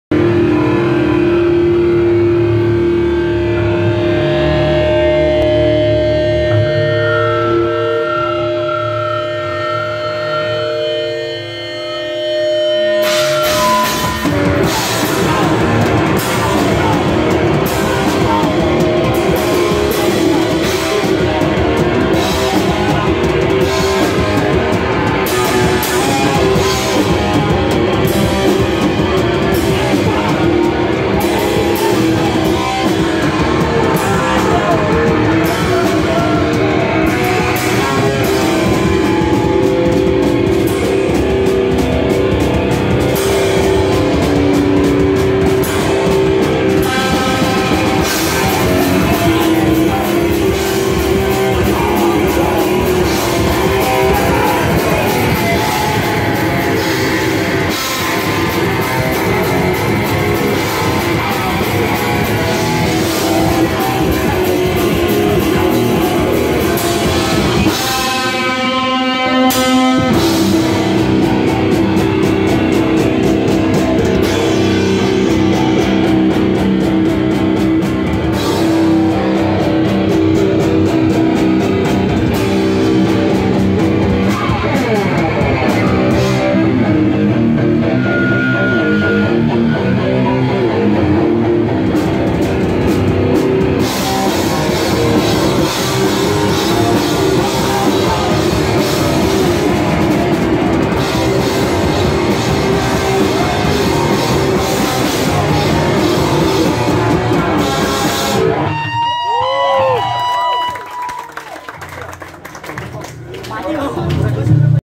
20220625 - Virtual Doom Part 2 from Gigs Akhir Bulan #10
Nama band: Virtual Doom
Kota: Yogyakarta
Acara: Gigs Akhir Bulan #10
Tanggal: 25 Juni 2022
Tempat: Warkop Hippies, Yogyakarta